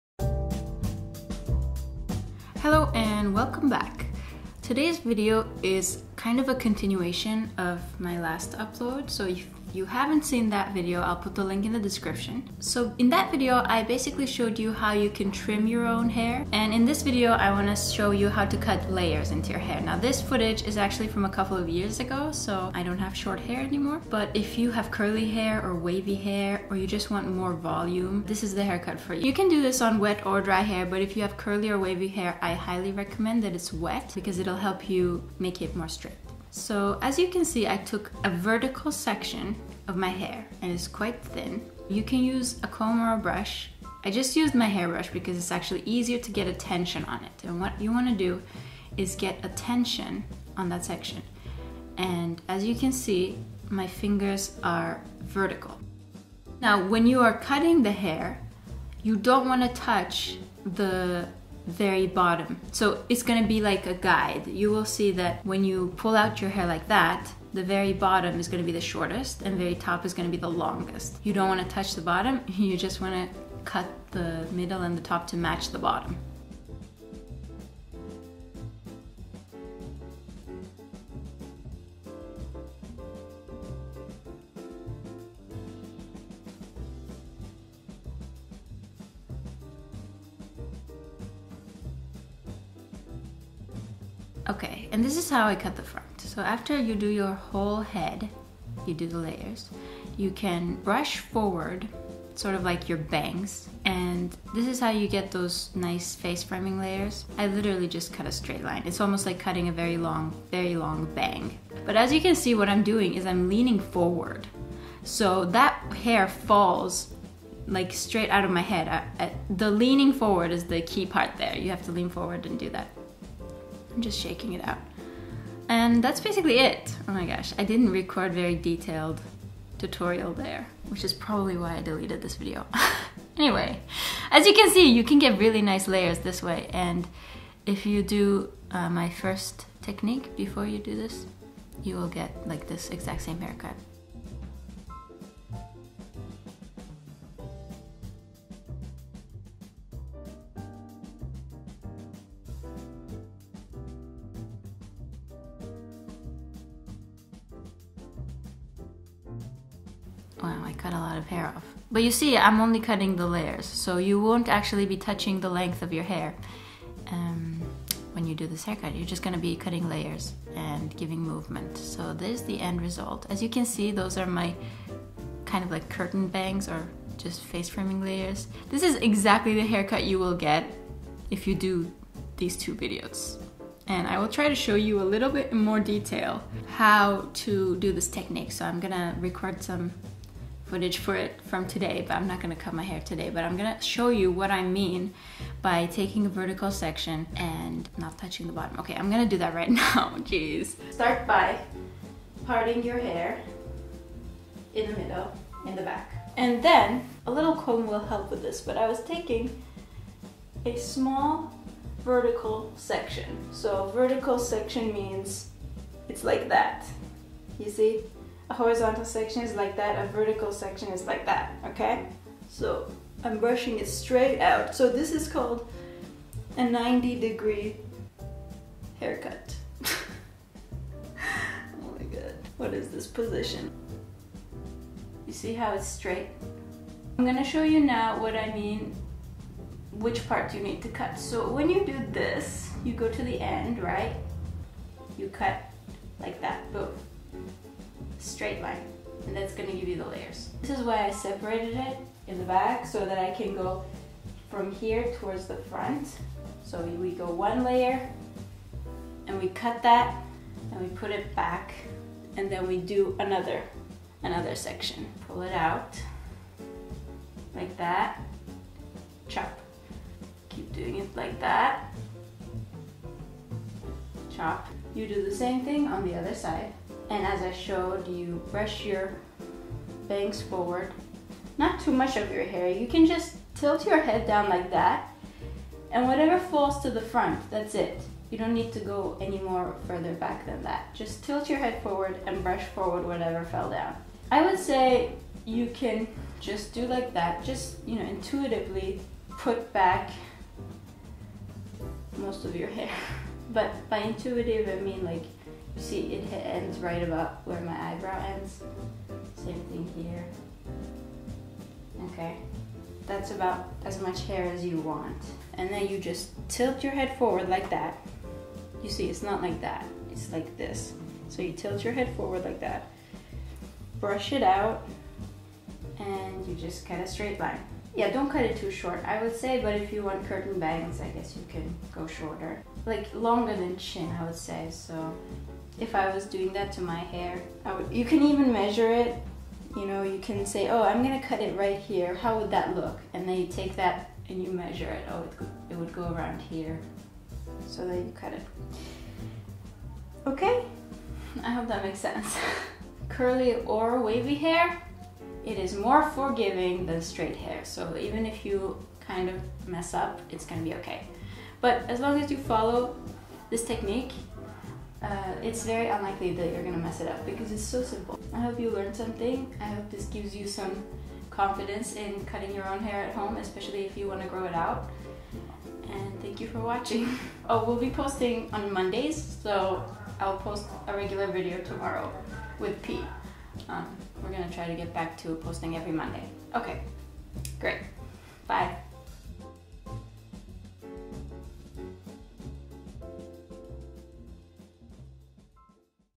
How To Cut Layers Into Your Own Hair
If you have wavy of curly hair, this is the perfect technique to give yourself a haircut. The first step, is giving yourself a trim by following my tutorial
And then just do this technique to layer your hair, for beautiful and voluminous waves!